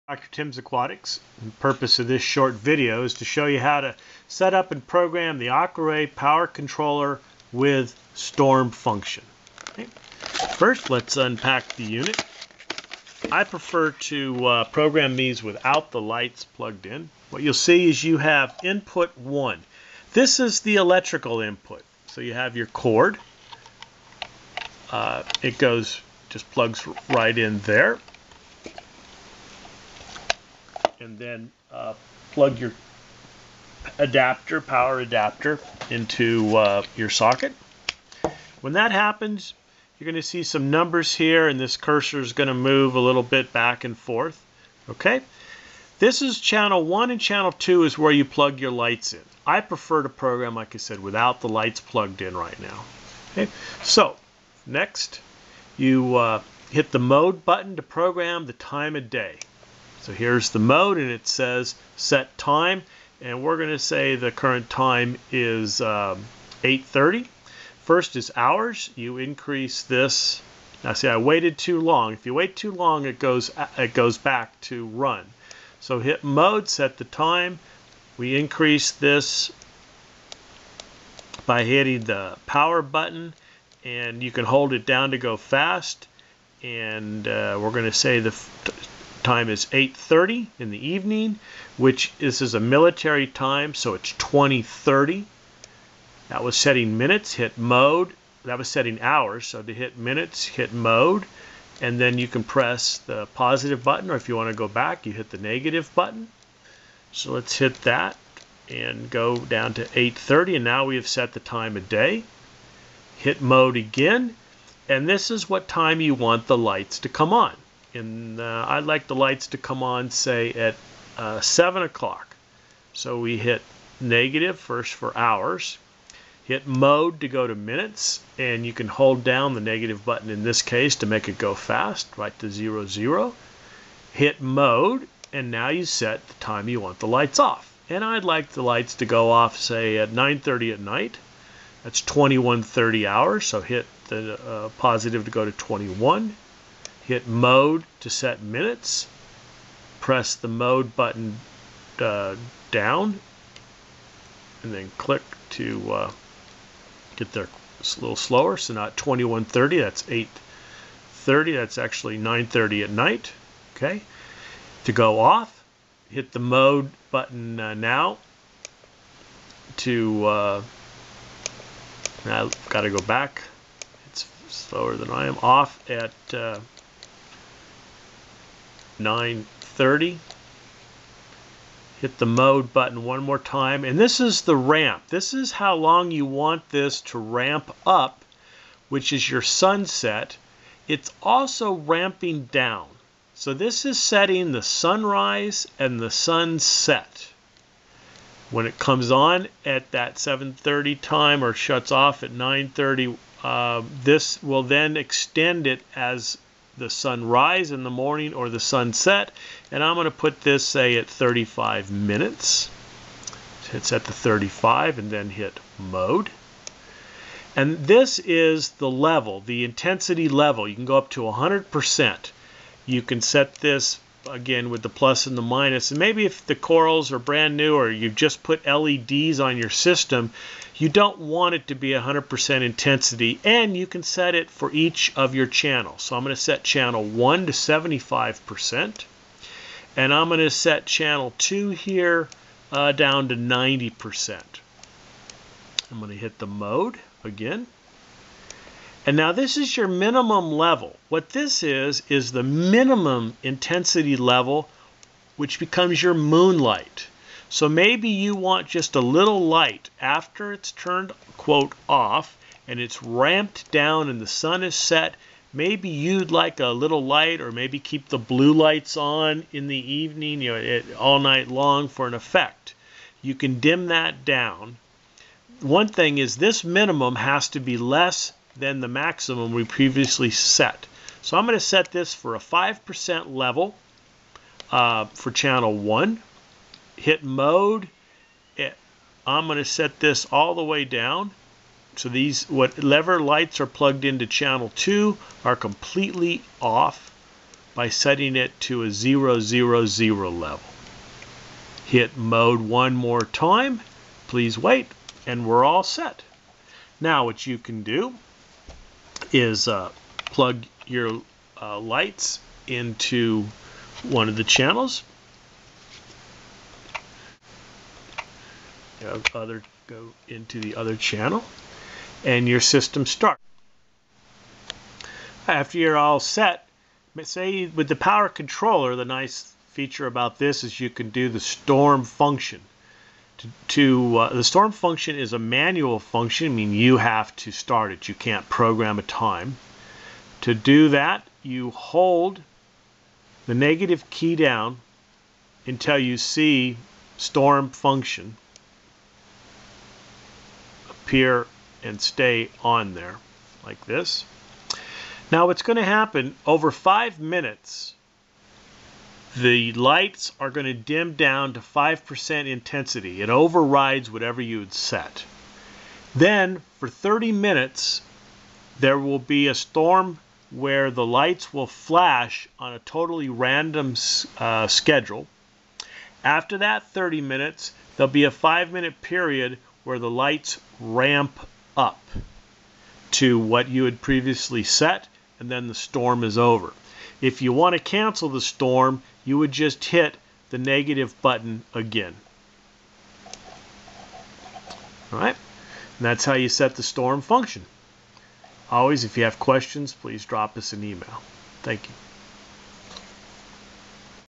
How to program the AquaRay LED Power Controller - DrTim's Aquatics
Video on how to program the AquaRay power controller with storm function (also applies to the AquaRay controller) for use with all AquaRay Aquarium LED light systems